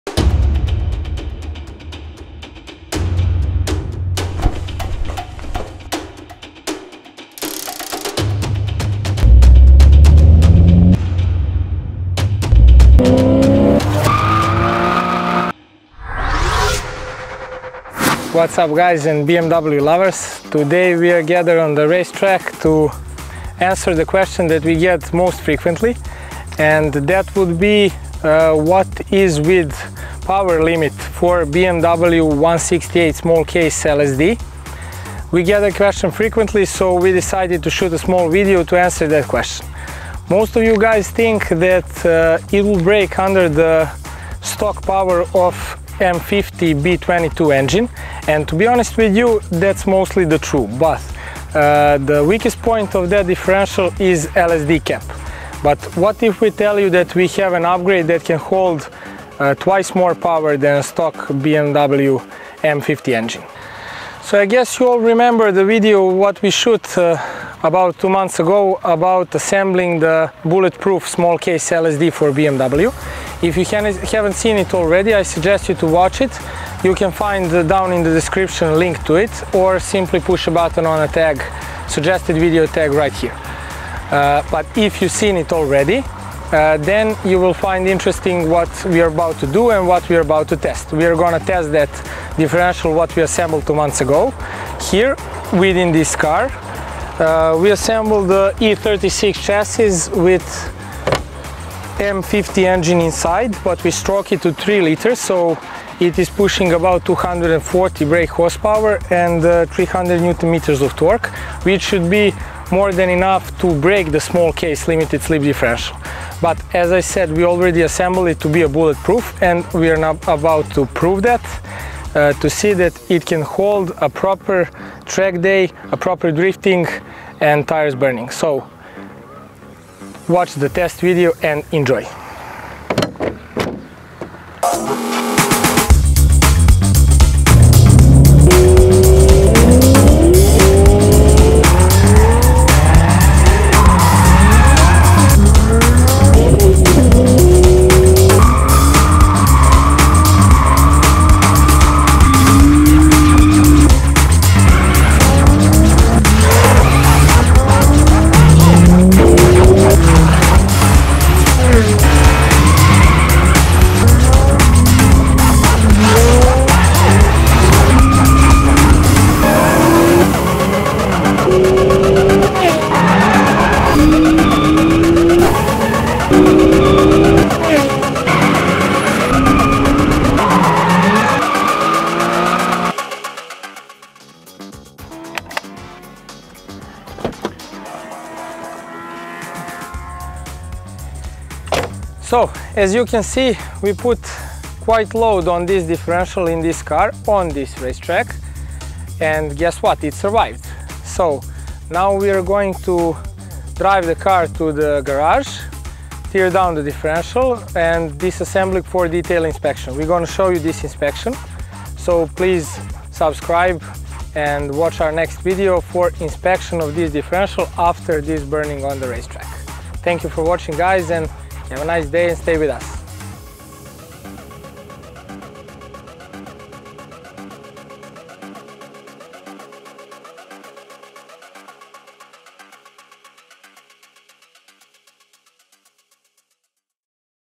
What is the real power limit of BMW E36 168mm small case LSD Differential?
I swapped M50B25 in my E30 318i, but can I keep the diff? Is it safe to drive small case Limited slip differential in my E36 328i? How much power can it handle?

If you have similar doubts, than this video is for you. In some of previous videos we showed you how to make a bulletproof 168mm LSD.

In this video, we are testing that very same differential. Make sure you subscribe to our channel so you don't miss our next video in which we are going to reveal the condition of internals after this 1 hour of rampage on the race track.